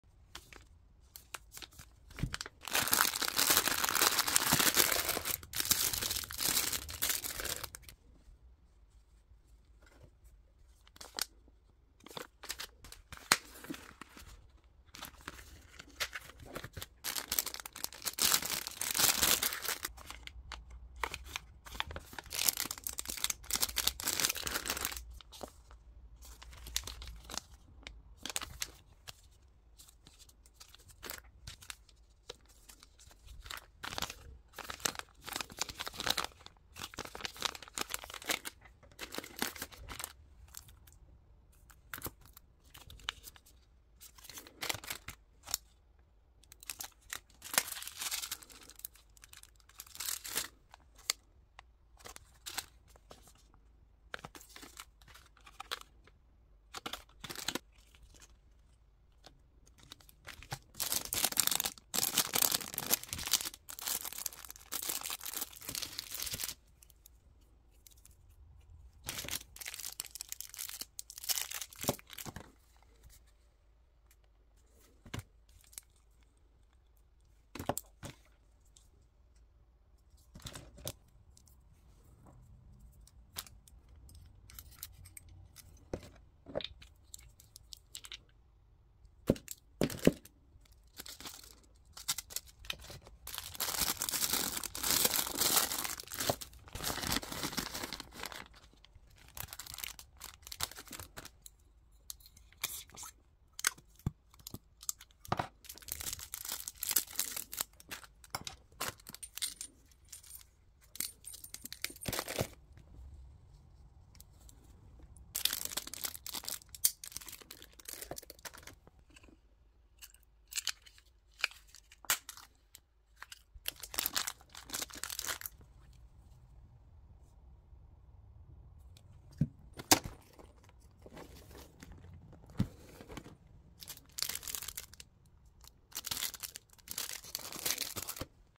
Everything Self-care Capsules for Elizabeth! Thank you for your order. ❤️ Link in bio to purchase!
Everything Self-care Capsules for Elizabeth!! I hope you enjoy everything and Thank you so much for your order.